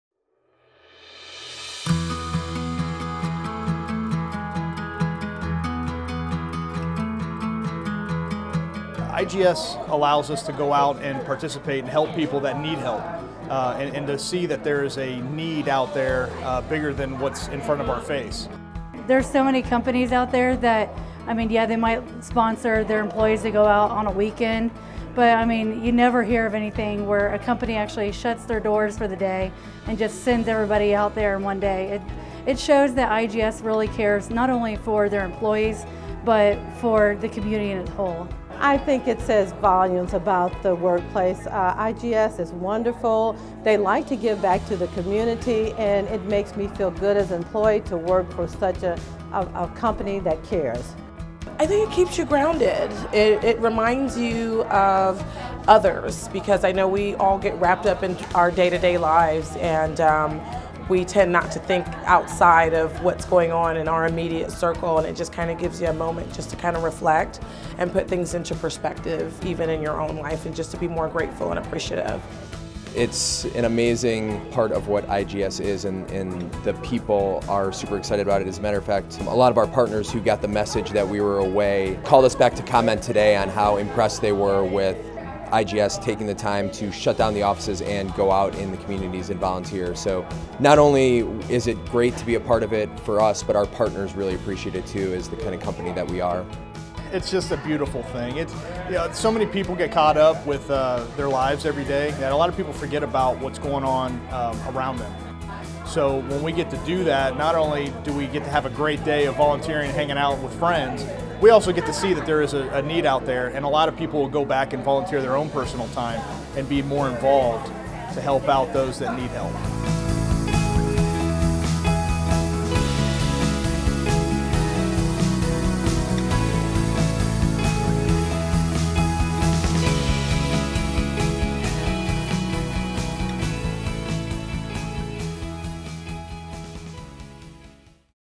IGS All-Employee Day of Service 2017 | IGS Impact
During the IGS All-Employee Day of Service, IGS closed its doors and employees spent the day serving in communities where they live and work. Watch how the day impacted IGS employees and their communities: